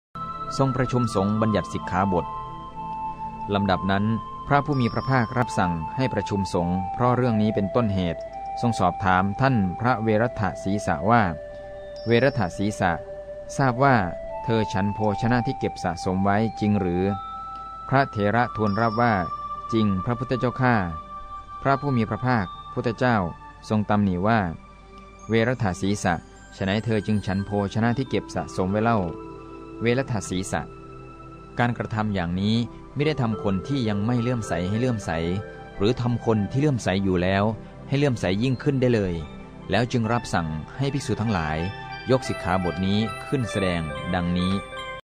02-535 ทรงประชุมสงฆ์บัญญัติสิกขาบท
ทรงประชุมสงฆ์บัญญัติสิกขาบท
            ลำดับนั้น    พระผู้มีพระภาครับสั่งให้ประชุมสงฆ์เพราะเรื่องนี้เป็นต้นเหตุทรง
สอบถามท่านพระเวฬัฏฐสีสะว่า    “เวฬัฏฐสีสะ    ทราบว่า    เธอฉันโภชนะที่เก็บสะสมไว้
จริงหรือ”    พระเถระทูลรับว่า    “จริง    พระพุทธเจ้าข้า”    พระผู้มีพระภาคพุทธเจ้า
ทรงตำหนิว่า    “ฯลฯ    เวฬัฏฐสีสะ    ไฉนเธอจึงฉันโภชนะที่เก็บสะสมไว้เล่า    เวฬัฏฐสีสะ
การกระทำอย่างนี้    มิได้ทำคนที่ยังไม่เลื่อมใสให้เลื่อมใส    หรือทำคนที่เลื่อมใสอยู่แล้วให้
เลื่อมใสยิ่งขึ้นได้เลย    ฯลฯ”    แล้วจึงรับสั่งให้ภิกษุทั้งหลายยกสิกขาบทนี้ขึ้นแสดงดังนี้